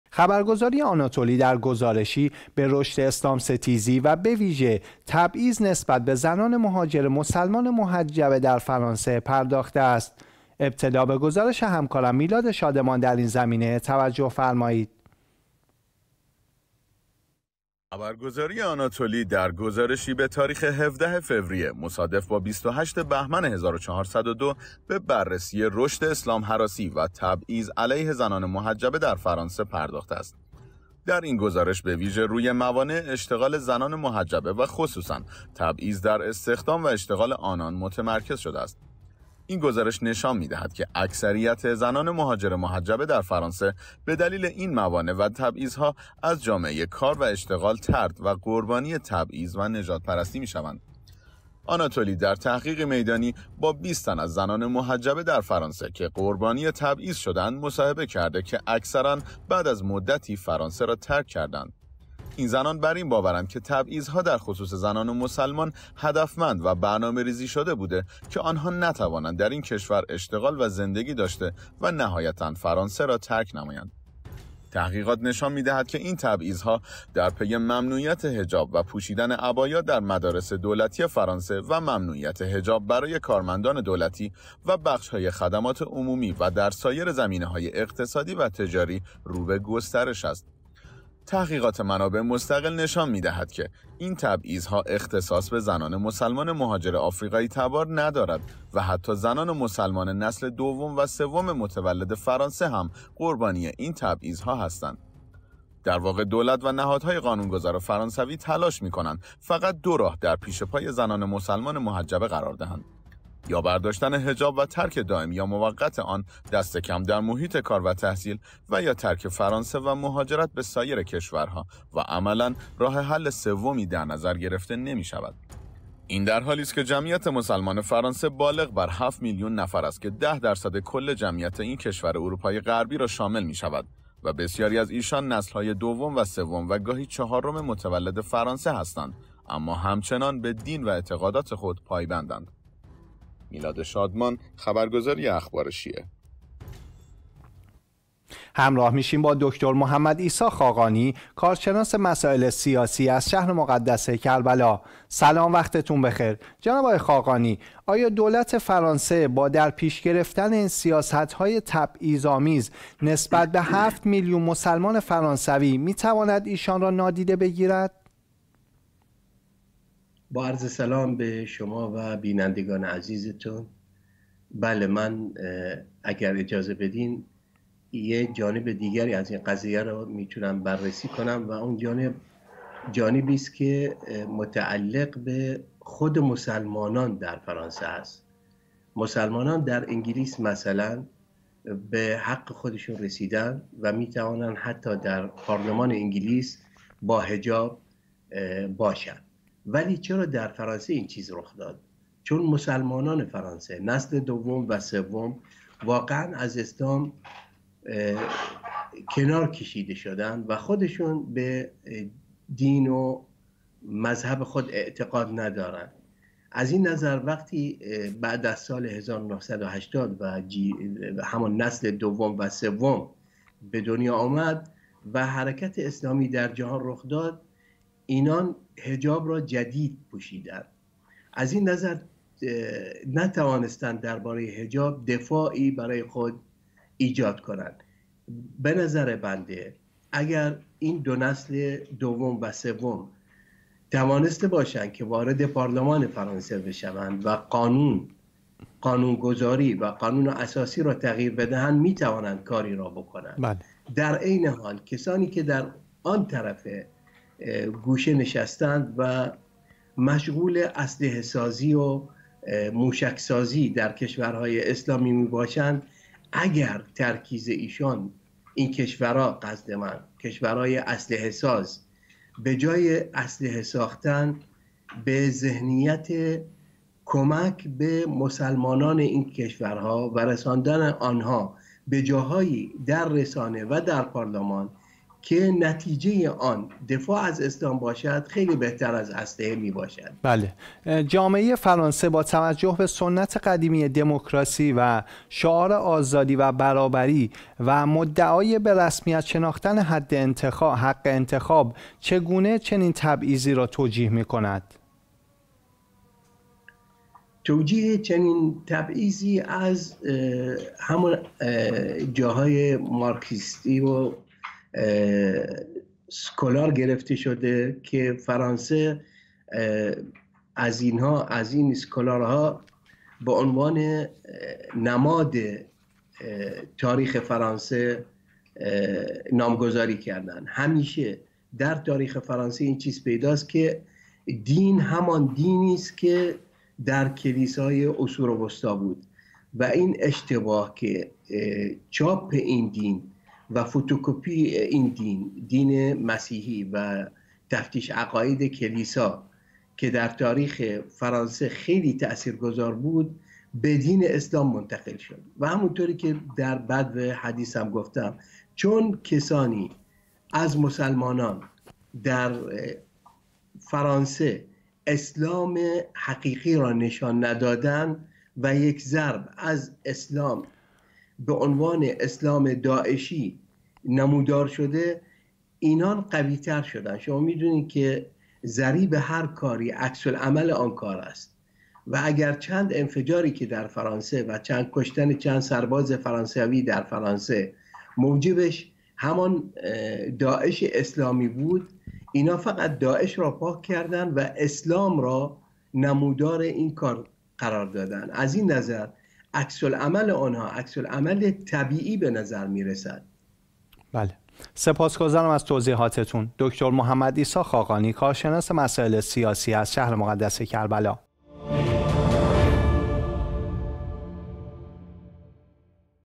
اسلام هراسی و تبعیض نسبت به زنان مسلمان در فرانسه
میلاد شادمان گزارش می دهد👉👉

گفتگوی اخبار شیعه با دکتر محمدعیسی خاقانی -
کارشناس مسائل سیاسی از شهر مقدس کربلا 🔸🔸
خبرگزاری آناتولی در گزارشی به رشد اسلام ستیزی و بویژه تبعیض نسبت به زنان مهاجر مسلمان محجبه در فرانسه پرداخته است

خبرگزاری آناتولی در گزارشی به تاریخ ۱۷ فوریه مصادف با ۲۸ بهمن ۱۴۰۲ به بررسی رشد اسلام  هراسی و تبعیض علیه زنان محجبه در فرانسه پرداخته است. در این گزارش بویژه روی موانع اشتغال زنان محجبه و خصوصا تبعیض در استخدام و اشتغال ایشان متمرکز شده است. این گزارش نشان می دهد که اکثریت زنان مهاجر محجبه در فرانسه به دلیل این موانع و تبعیض ها از جامعه ی کار و اشتغال طرد و قربانی تبعیض و نژاد پرستی می شوند.
آناتولی در تحقیقی میدانی با ۲۰ تن از زنان محجبه در فرانسه که قربانی تبعیض شده اند مصاحبه کرده که اکثرا بعد از مدتی فرانسه را ترک کرده اند. این زنان بر این باورند که تبعیض ها در خصوص زنان مسلمان هدفمند و برنامه ریزی شده بوده که ایشان نتوانند در این کشور کار و اشتغال و زندگی داشته و نهایتا فرانسه را ترک نمایند.
تحقیقات نشان می دهد که این تبعیض ها در پی ممنوعیت حجاب و پوشیدن عبایا در مدارس دولتی فرانسه و ممنوعیت حجاب برای کارمندان دولتی و بخش های خدمات عمومی رو به گسترش در سایر بخش های اقتصادی و تجاری است.
تحقیقات منابع مستقل نشان می دهد که این تبعیض ها اختصاص به زنان مسلمان مهاجر افریقایی تبار ندارد و حتی زنان مسلمان نسل دوم و سوم متولد فرانسه هم قربانی این تبعیض ها هستند.
در واقع دولت و نهادهای قانونگذار فرانسوی تلاش می کنند فقط دو راه در پیش پای زنان مسلمان محجبه قرار دهند. یا برداشتن حجاب و ترک دائم و یا موقت آن دستکم در محیط کار و تحصیل و یا ترک فرانسه و مهاجرت به سایر کشورها و عملا راه حل سومی در نظر گرفته نمی شود.
این در حالی است که جمعیت مسلمانان فرانسه بالغ بر ۷ میلیون نفر است  که ۱۰ درصد کل جمعیت این کشور اروپای غربی را شامل می شود و بسیاری از ایشان نسل های دوم  سوم و گاهی چهارم متولد فرانسه هستند اما همچنان به دین و اعتقادات خود پایبندند.